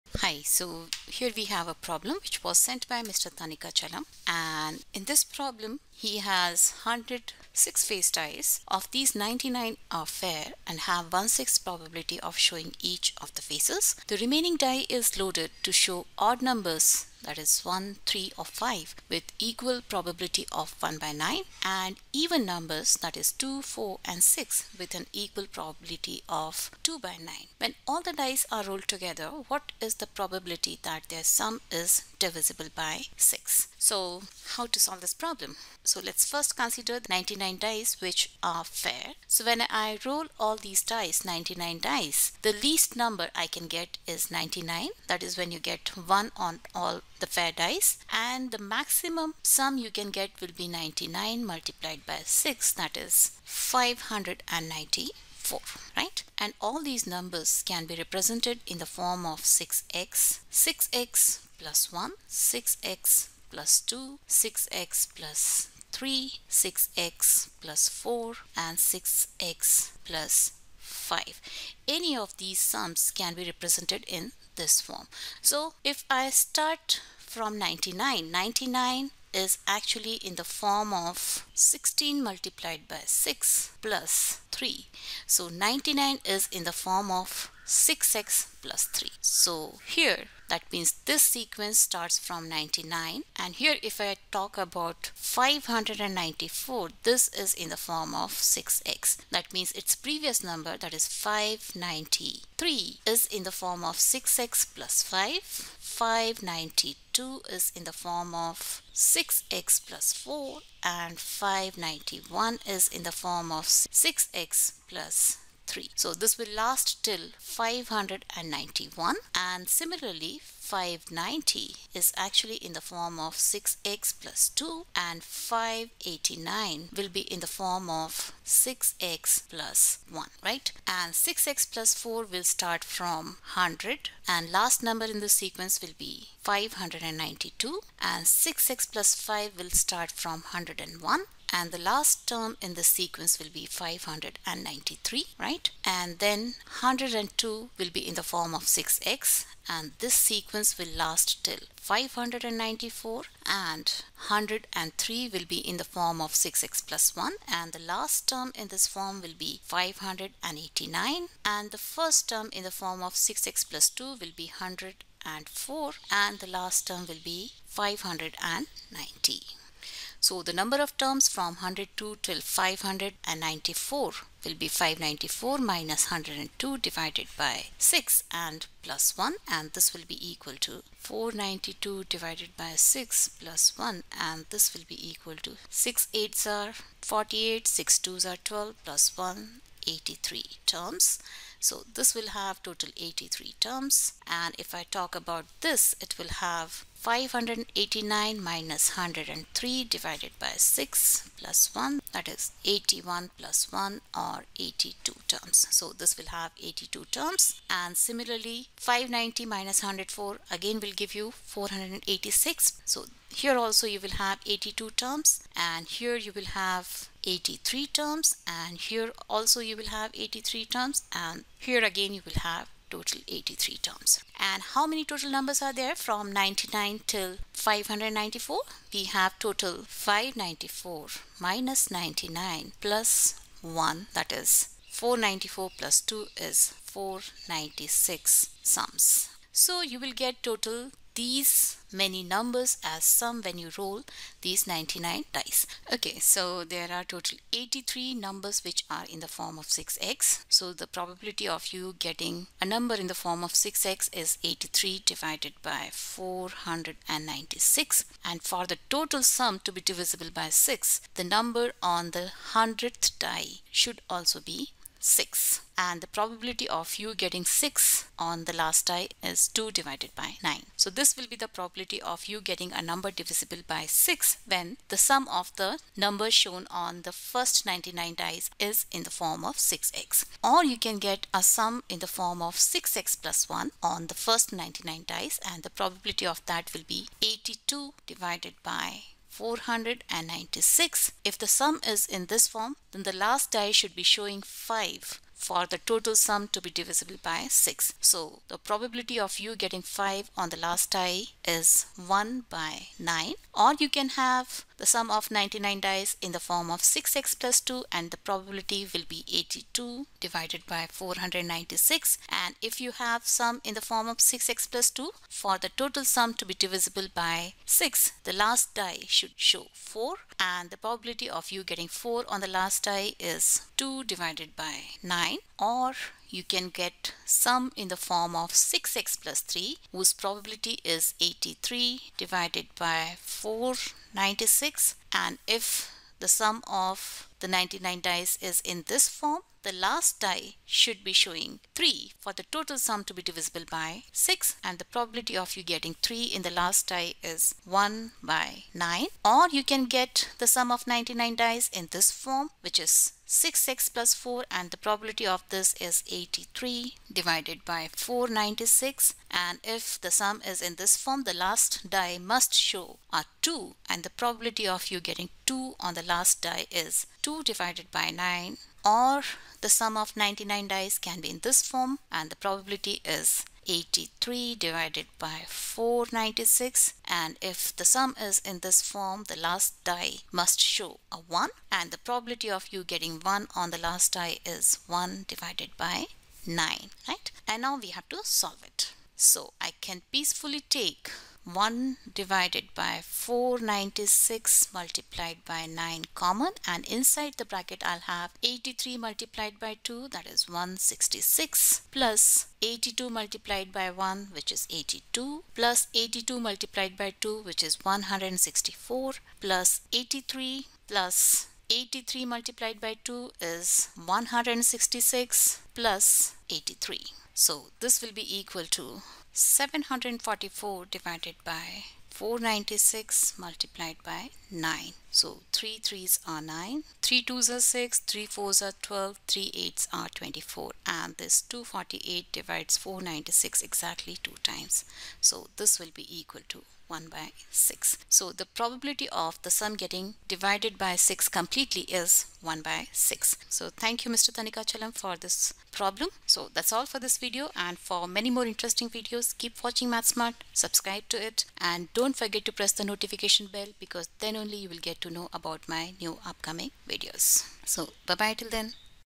Probability | 99 fair dice and 1 faulty die, Find the probability of sum divisible by 6 Hard problem
I have 100 six face dice. Of these 99 are fair and have 1/6 probability of showing each face. The remaining die is loaded to show odd numbers 1,3 or 5 with equal probability of 1/9 and even number 2,4 or 6 with an equal probability of 2/9. When the dice are rolled together what is the probability, that the sum is divisible by 6?